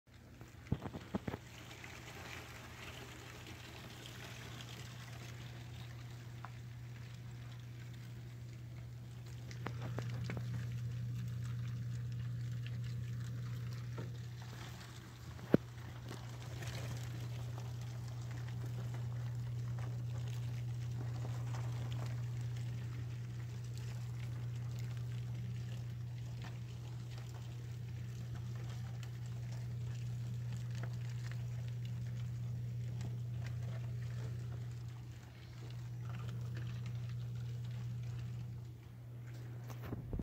Dubia roach colony
My intrusion of roaches. Started with 2000 tiny babies in April. Now they are grown and producing tons of babies on their own.